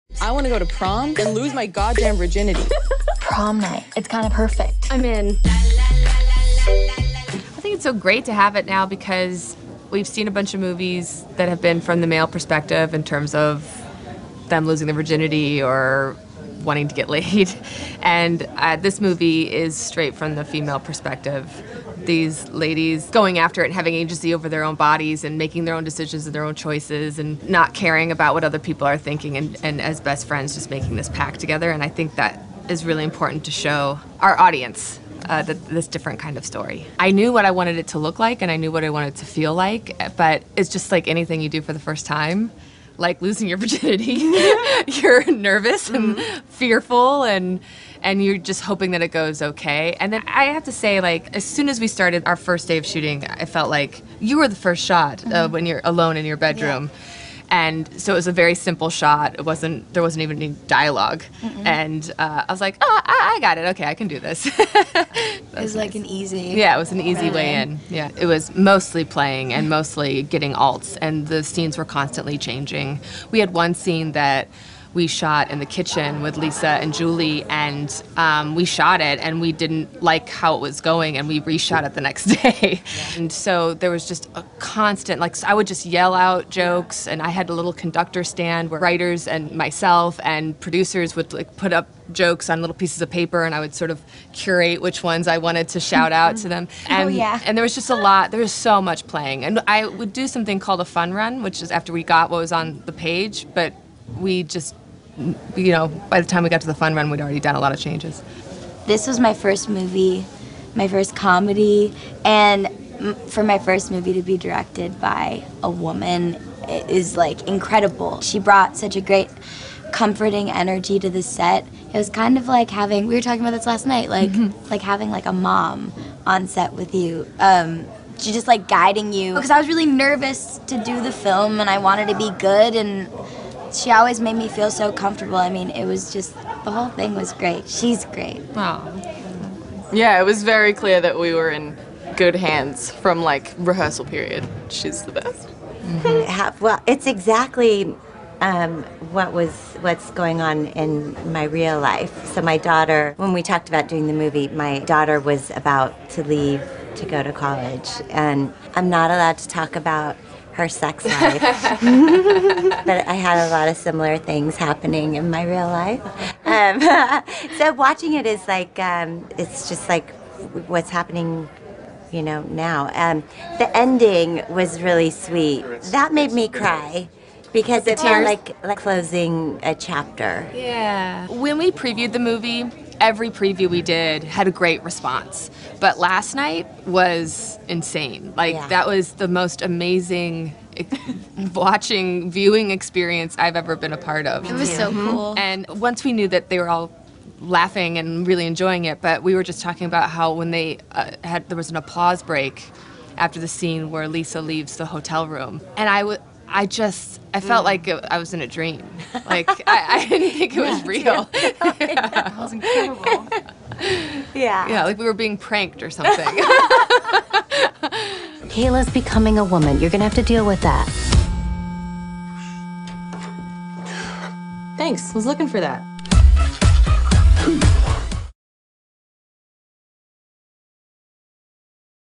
Kay Cannon and the cast of Blockers talk flipping the teen sex comedy on its head
After serving as screenwriter and producer on 30 Rock, New Girl, and the Pitch Perfect films, Kay Cannon makes her directorial debut this Friday with Blockers, and early reviews are pretty positive. The film follows in the footsteps of teen sex comedies like American Pie, or Superbad, but tells its story from a female perspective. Blockers centers on a sex pact made by three teenage girls, and their parents who find out about the pact and attempt to put a stop to it. At the SXSW premiere of the film, we spoke with Cannon, Leslie Mann, Geraldine Viswanathan, and Gideon Aldon about the film.